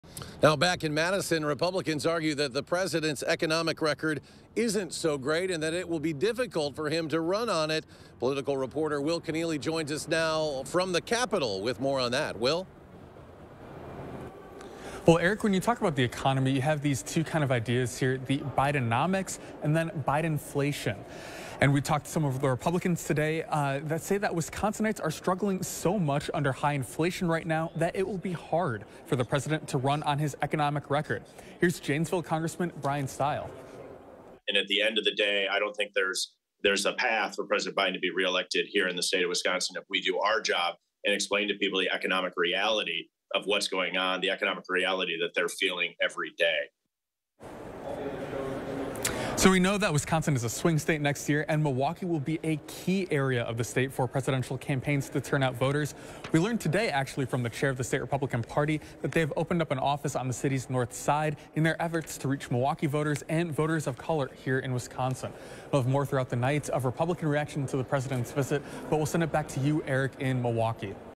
GOP reacts to Biden's visit to Milwaukee, blasting 'Bidenomics'
Republicans from Wisconsin argue that it will be hard for President Joe Biden to carry Wisconsin in the 2024 election due to his economic policies.